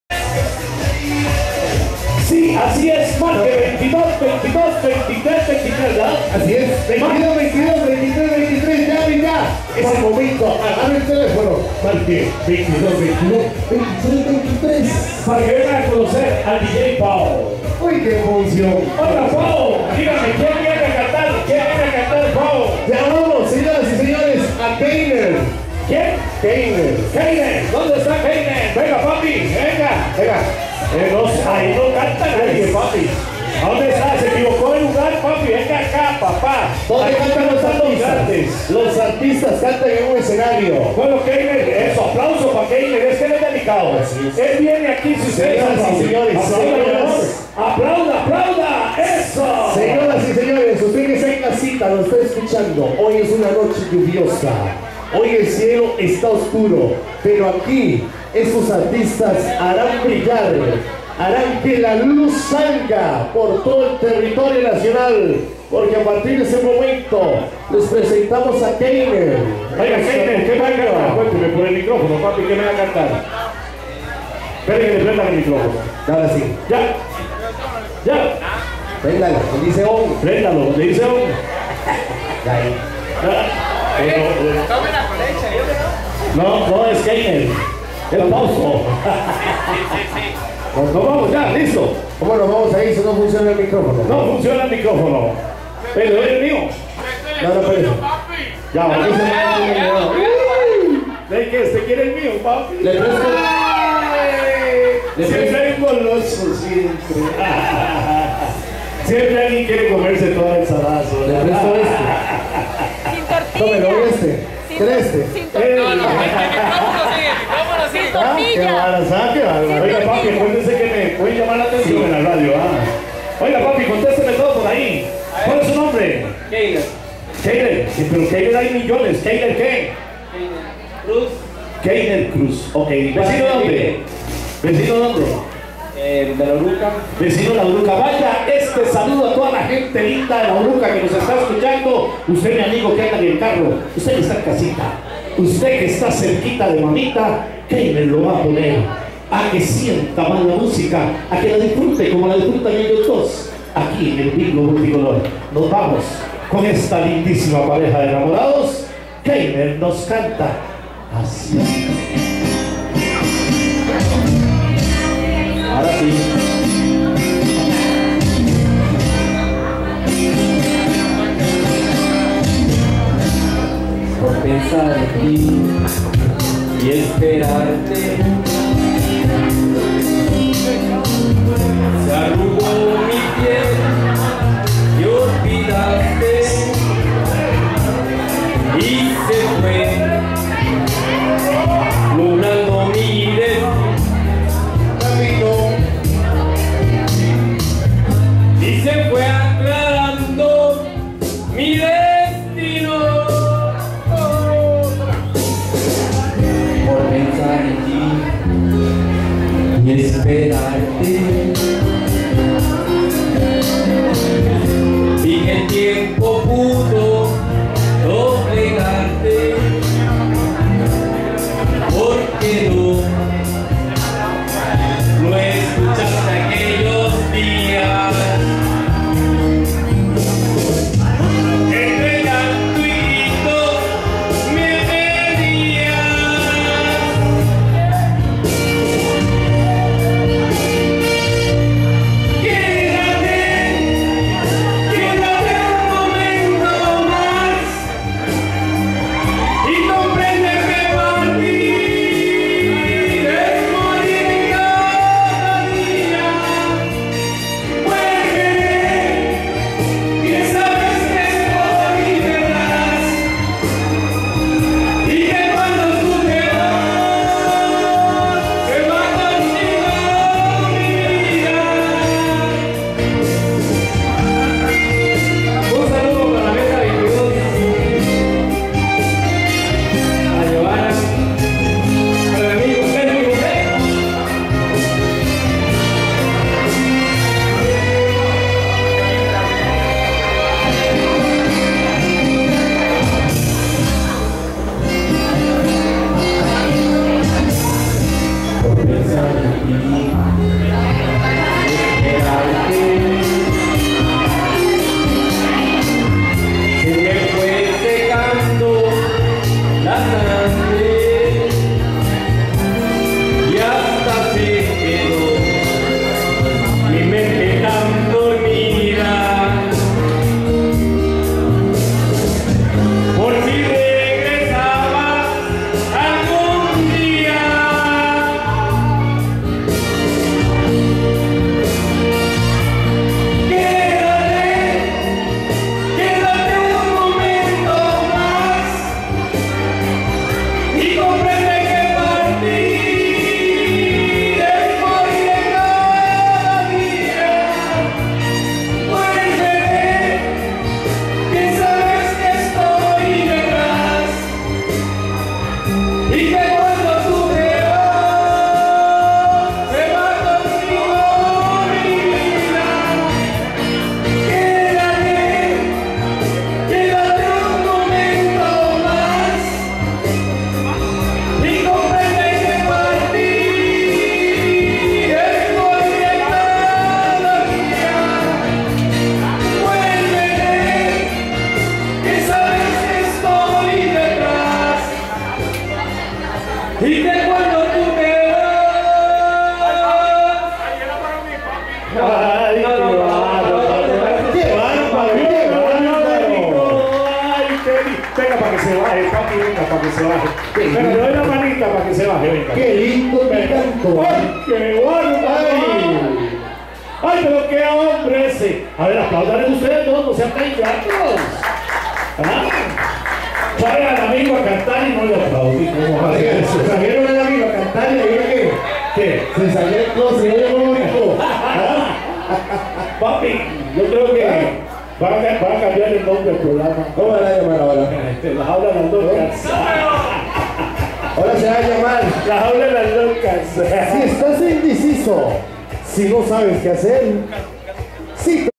Heyner Canta desde el Bingo Multicolor llame al 2222-2323
Saludos los esperamos recuerde que canta en radio en el programa zitro tu karaoke puede clasificar al campeonato nacional de karaoke llega temprano cantan 5 y el ganador  se gana 25000 en efectivo